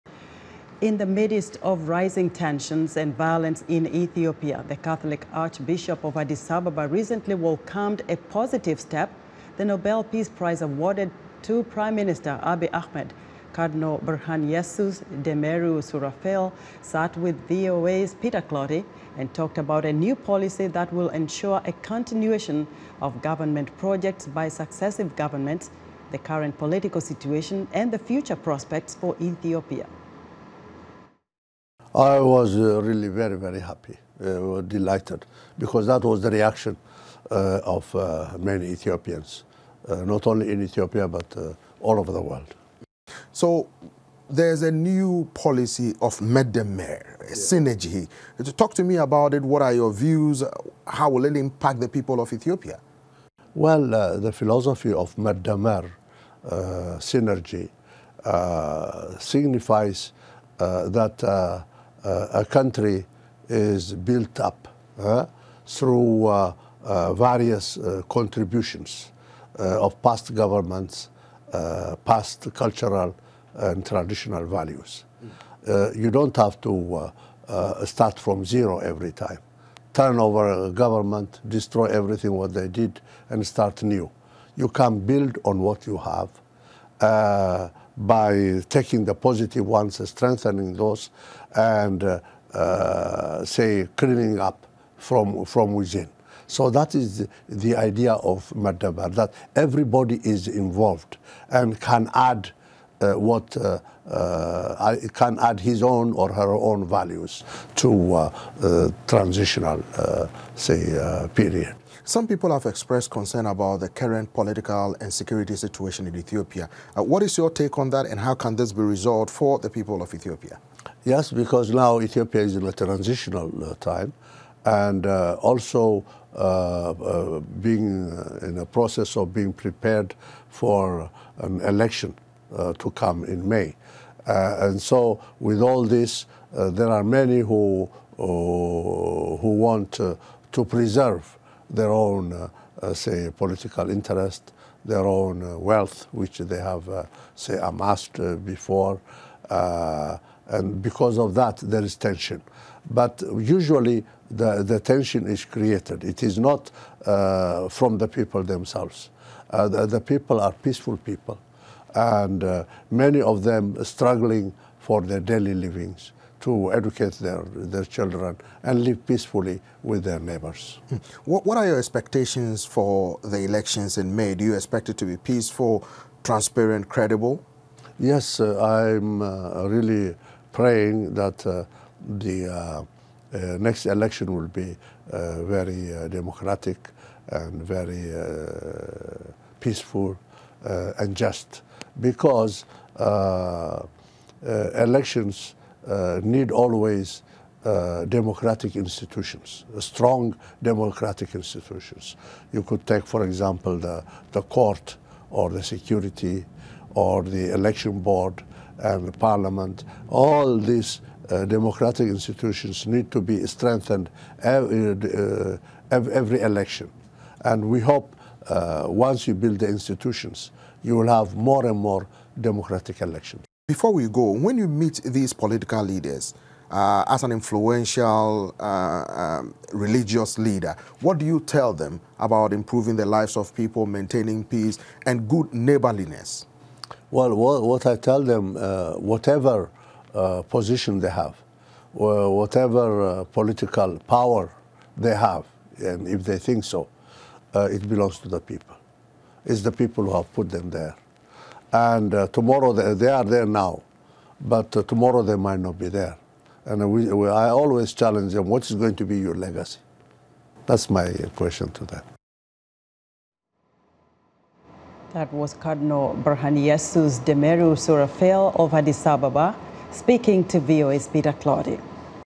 Cardinal Berhane-Yesus Demerew Souraphiel
In the midst of rising tensions and violence in Ethiopia, The Catholic Archbishop of Addis Ababa recently welcomed as a positive step --the Nobel Peace Prize awarded to Prime Minister Abiy Ahmed. Cardinal
Berhane-Yesus Demerew Souraphiel sat with VOA’s Peter Clottey and talked about a new policy that will ensure a continuation of government projects by successive governments, the current political situation and the future prospects for Ethiopia.